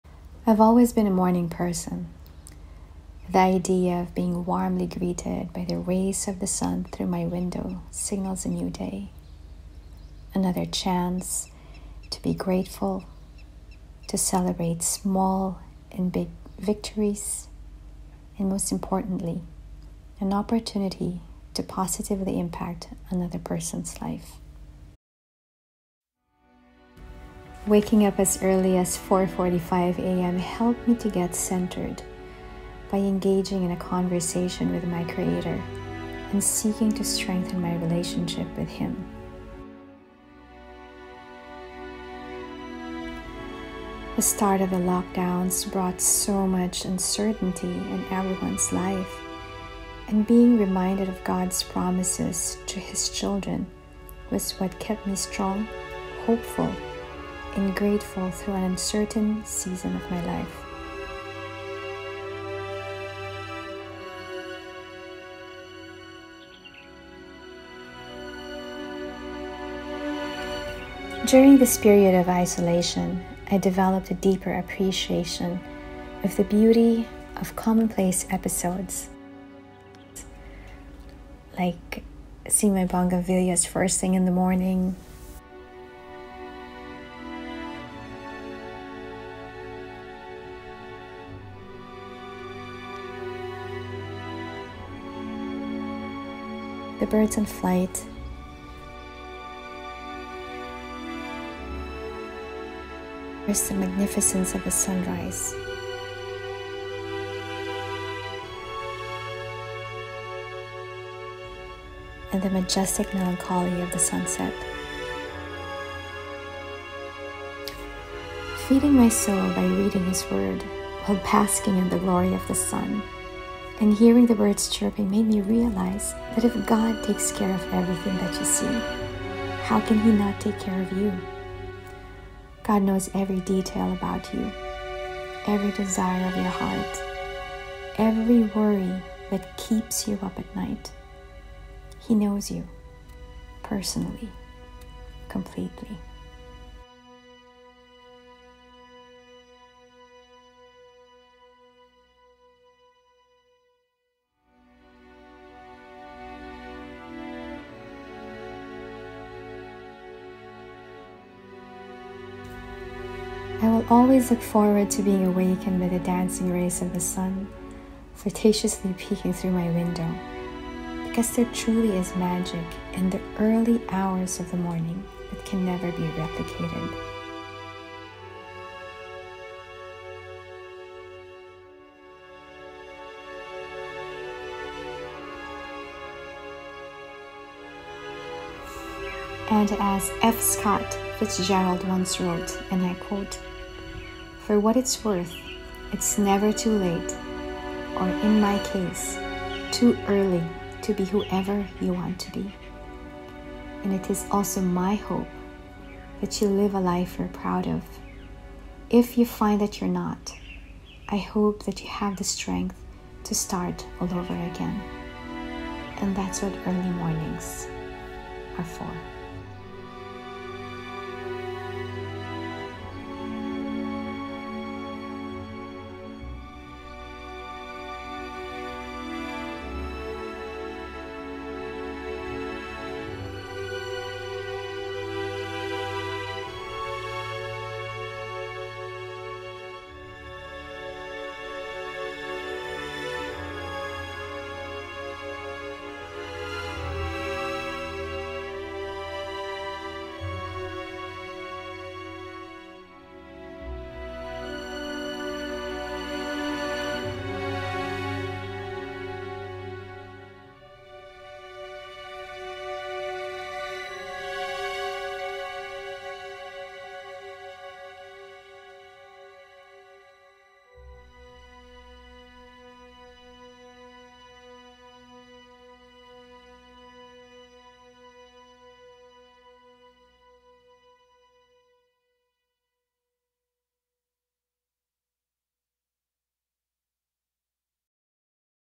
THE GIFT OF MORNINGS
I have always been a Morning person. The idea of being warmly greeted by the rays of the sun through my window signals a new day - another chance to be grateful, to celebrate victories whether they are small or big and most importantly an opportunity to positively impact another person's life.

I hope that this Monday video will inspire all of us to see the beauty in the ordinary, to be grateful for the life that we get to live every second of each day and to grab every opportunity to positively impact the lives of the people that we meet.

Have a blessed week everyone and live it up.

Always,
Anna